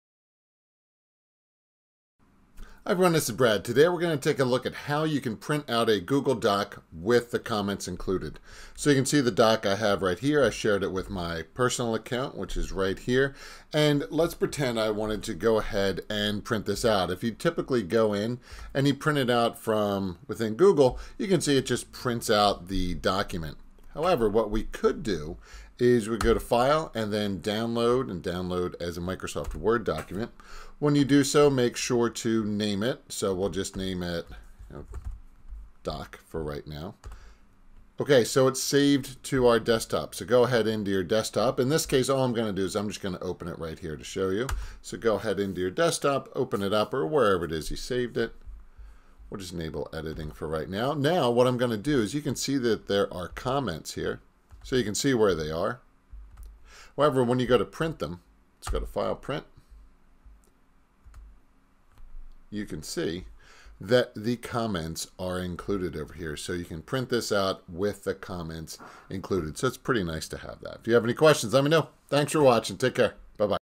How to Print a Google Doc with comments
If you use Google Docs and would like to print out the comments that exist on the document try the following steps.
Make sure to go to file, choose Download, make sure to download as a Microsoft Doc (make sure to know where you doc saved - in my case it is the desktop), open the file and then choose to print. You will now see the document as well as the comments.
** If you use OpenOffice Write, be sure to choose that program from the “Open With” menu.